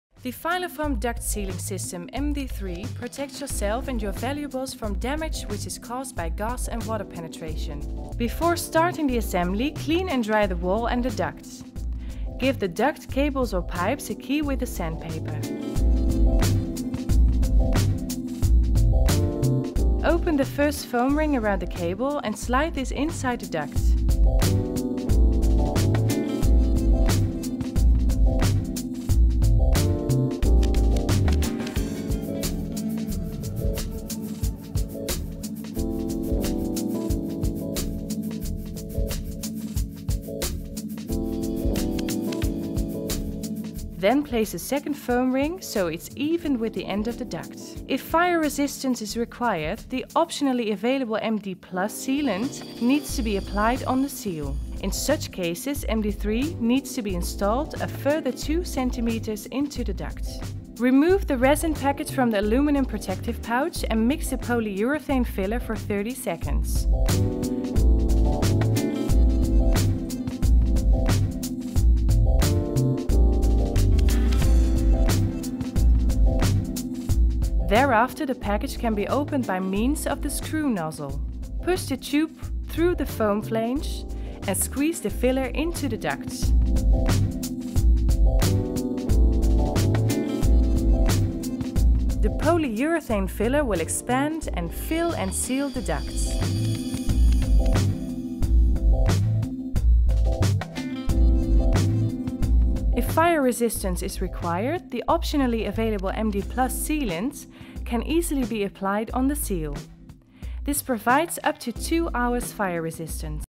Filoform MDIII Duct Seals - Watertight & Gastight Cable Duct Sealing Systems
Filoform MDIII Cable Duct Sealing System - Filoform MDIII cable duct sealing systems have been developed to provide effective and simple sealing solution for gas and watertight sealing of cables / pipes in ducts or bore holes. Filoform MDIII cable duct sealing system is extremely flexible and can be installed in ducts vertically and horizontally.

 Filoform Cable Duct Seal Systems

* Resistance to high levels of water and gas pressure

* Fire resistant cable duct seals, up to 2 hours - tested to BS EN 1366-3 : 2009

* Suitable for range of cable duct diameters and shapes

* Quick and easy to install duct seals

* Resists movement, shock and vibration

* Suitable for cable duct renovation and diversions - duct seals can be installed retro-fit

* ATEX compliant duct seals

* WIMES compliant duct seals (3.02 Clause 7.4.3.2.d&e)